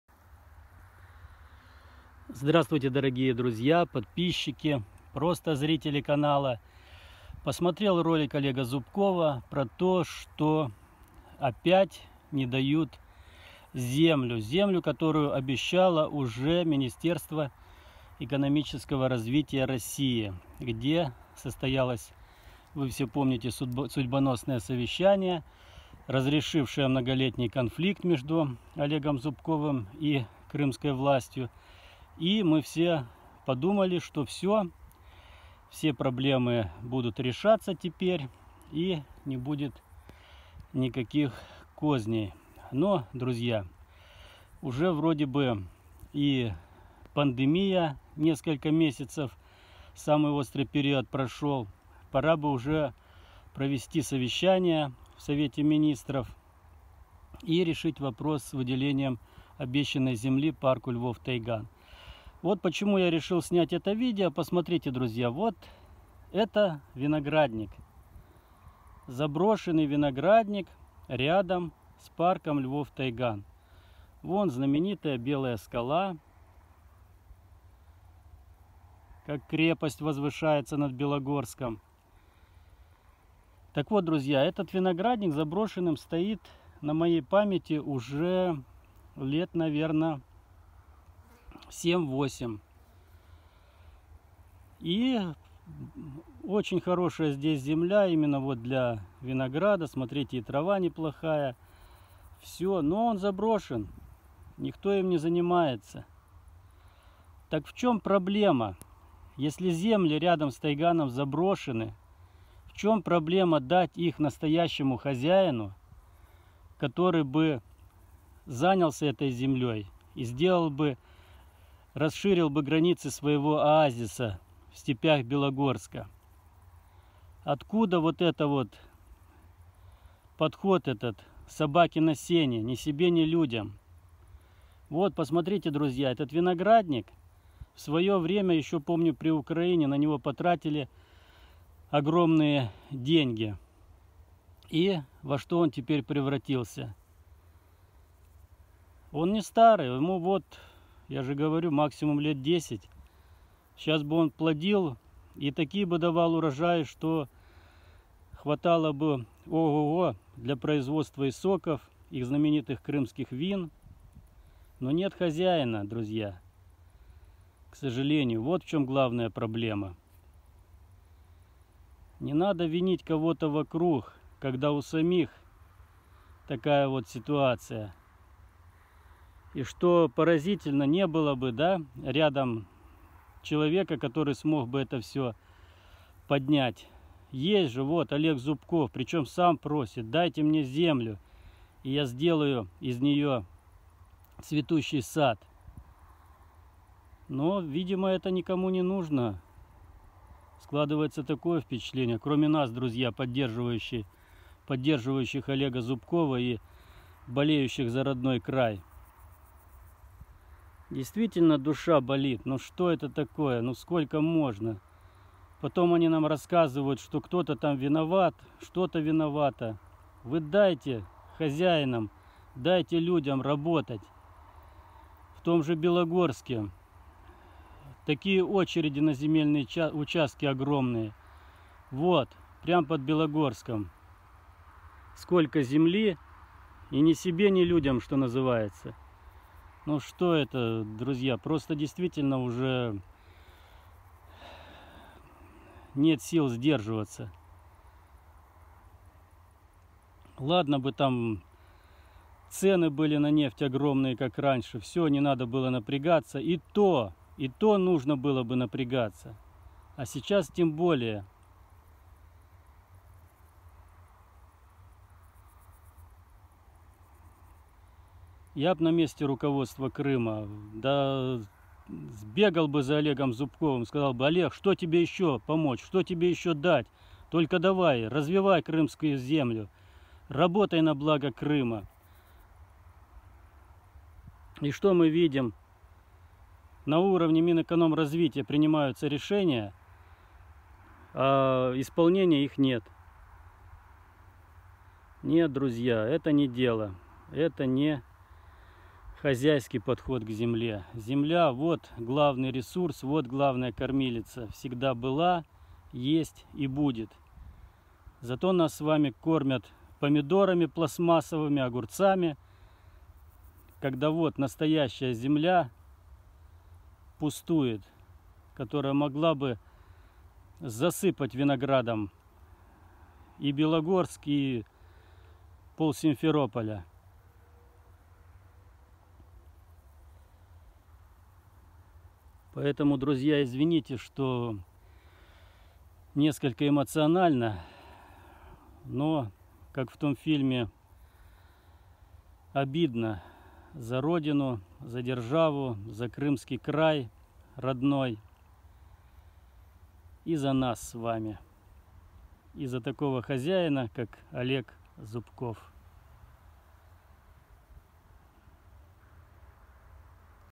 Обидно за Олега Зубкова и Крым!
Друзья, посмотрел видео, где Олег Зубков рассказывает о ситуации с выделением земли и  стало очень обидно, почему так происходит? Почему не дают развиваться на благо Крыма?